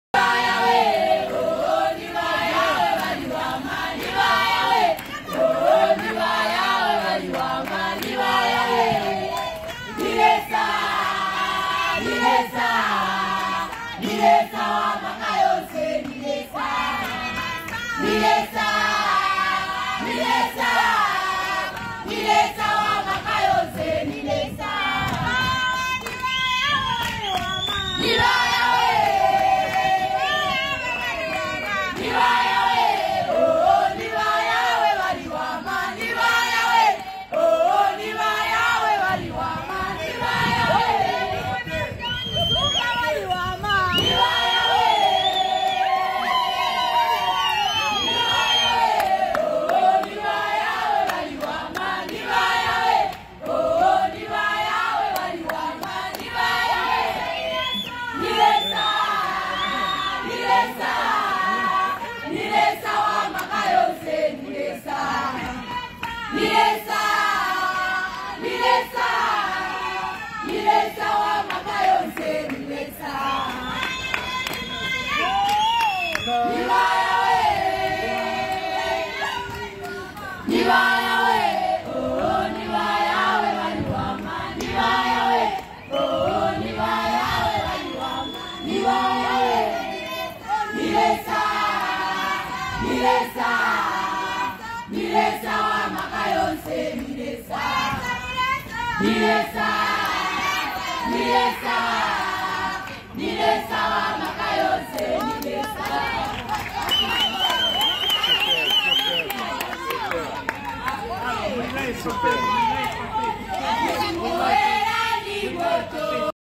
SHEPOLOPOLO⚽ CELEBRATE THEIR VICTORY OVER SENEGAL IN STYLE⚽
The Zambian women today beat Senegal women in penalties 4:2. And they have qualify to the Women World cup of nation for 2023.